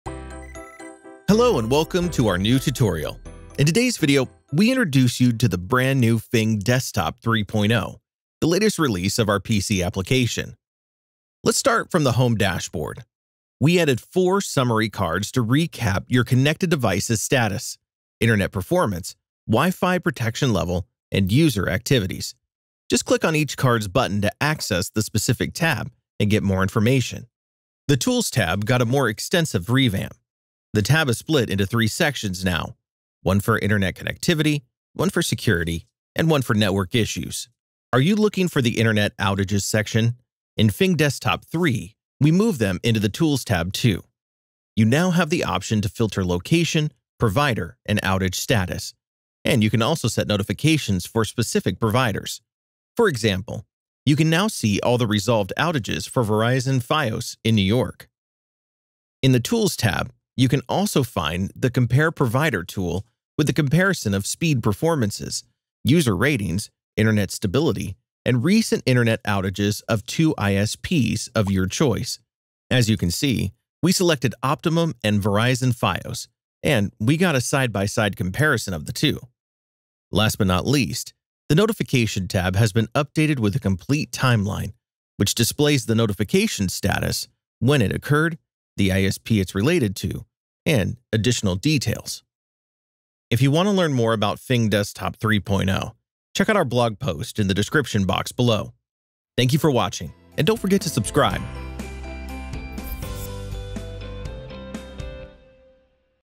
Fing Desktop 3.0 is here! New interface and navigation
Fing Desktop 3.0 is here! And it comes with a new interface, revamped tabs, and effortless navigation.

We rethought the user interface, with major changes on the home page, in the tools tab, and in the internet outages section. Take a look at what’s new, and get started with your new Fing Desktop experience!

Are you looking for a more detailed explanation?